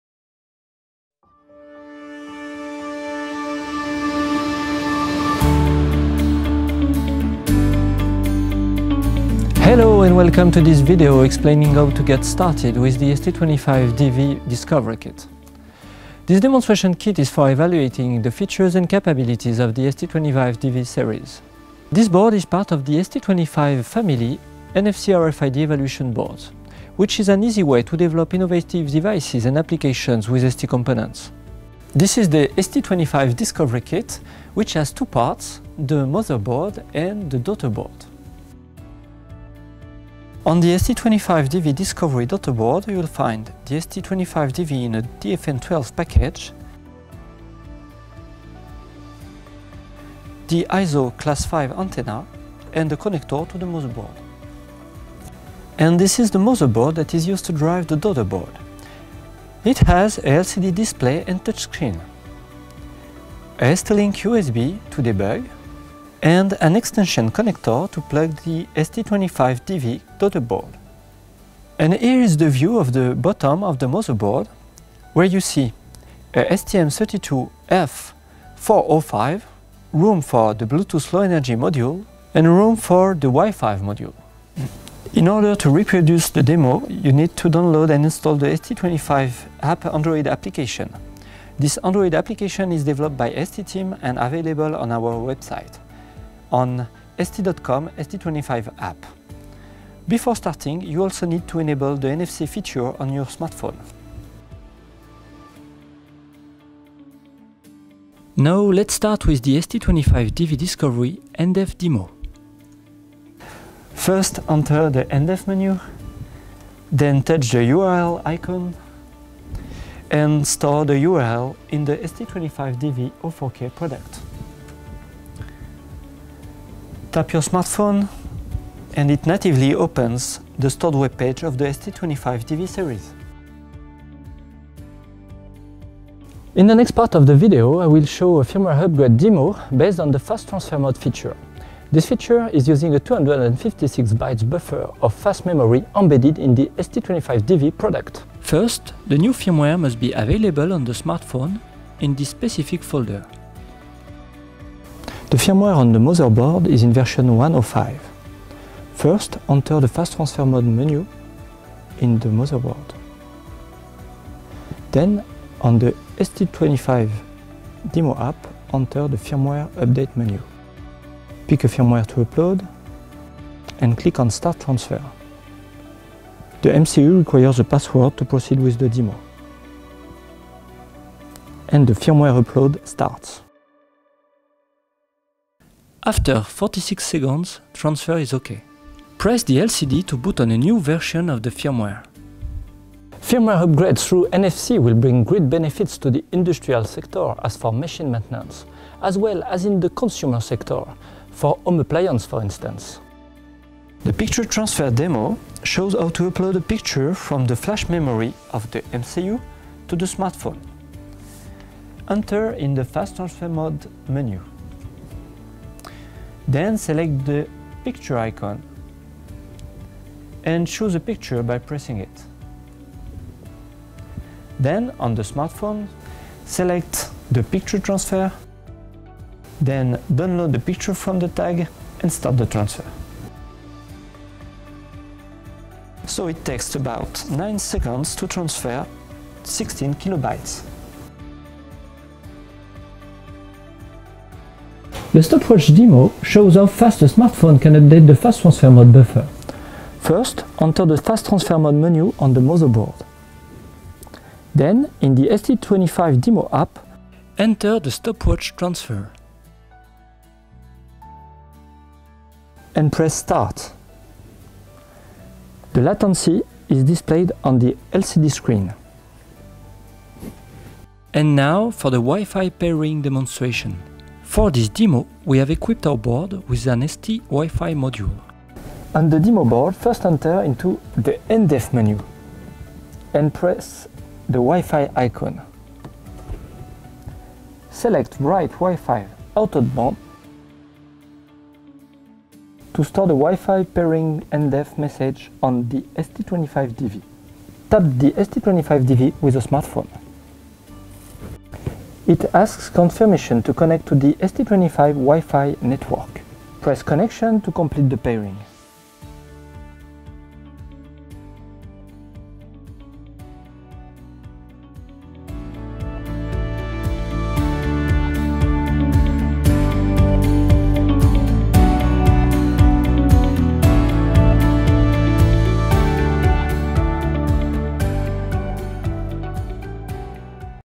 Getting Started NFC: ST25DV discovery kit smartphone demo
For your smartphone, download our free android app or you may also download it from google playstore by typing ST25 and selecting the ST25 NFC application.

In our short video, Remi goes quickly through a number of demonstrations with our ST25DV-discovery kit and a smartphone. The ST25DV-discovery kit is part of our ST25 NFC RFID evaluation boards.

The demos include:
- NDEF demo – URL link
- Firmware upgrade using the "Fast Transfer Mode" feature
- Picture transfer from MCU of discovery kit to smartphone
- Stopwatch demo to show how fast the smartphone can update the MCU
- Wifi pairing

Applications include: industrial for machine maintenance or consumer for home appliance SW upgrade for example.